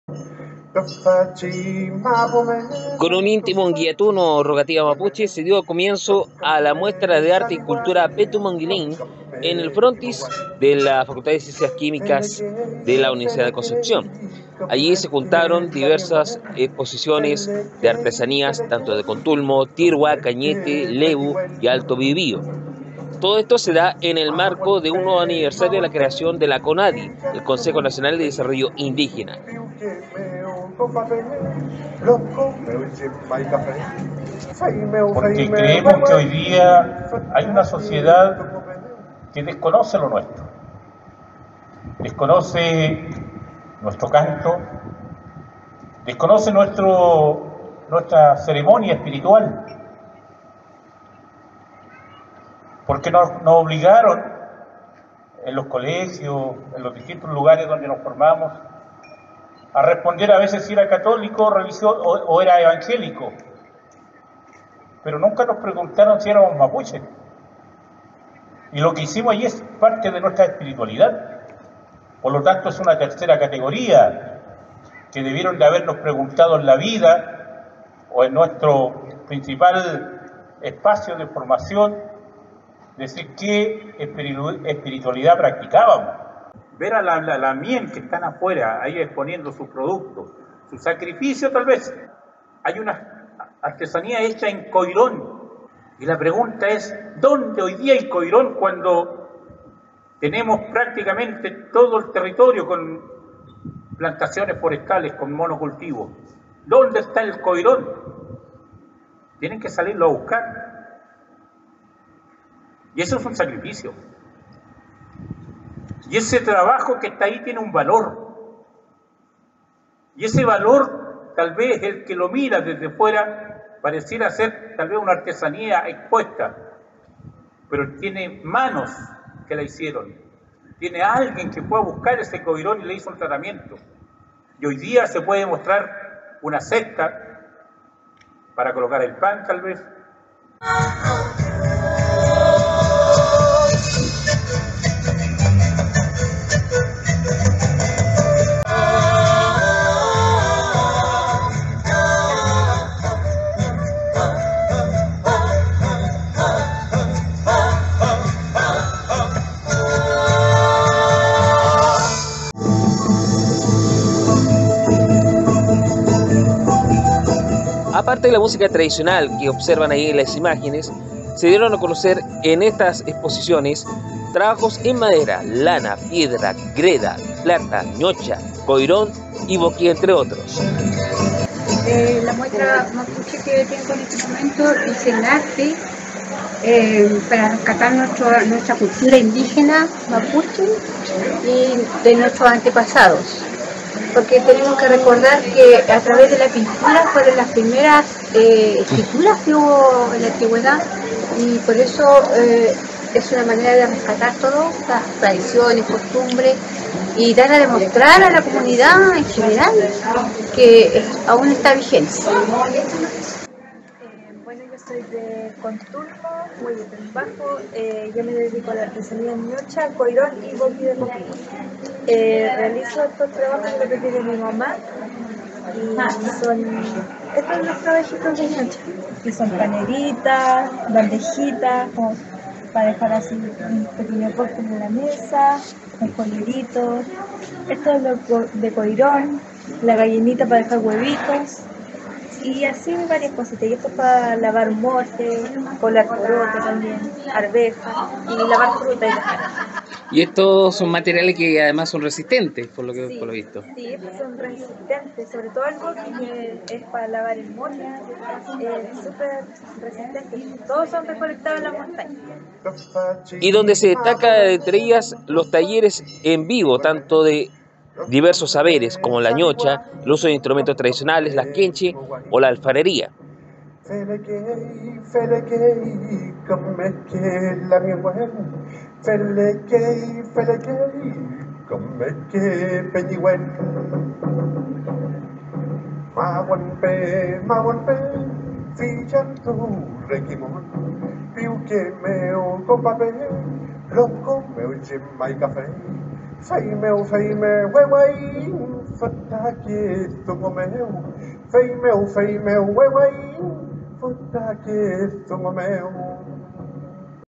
Vistazo Nota Feria Petu Mongeleiñ Concepción - 18OCT2024
22 expositores de diversos territorios de la región del Bio Bío son parte de la realización de la Feria de artesanías, saberes y lengua mapuche Petu Mongeleiñ en la Facultad de Ciencias Químicas de la UdeC y patrocinada por la CONADI.

Nota subida solo para fines informativos, sin ánimo de lucro y bajo reglas de uso justo de YouTube.
2024 - Asociación Audiovisual Digital El 3 de Conce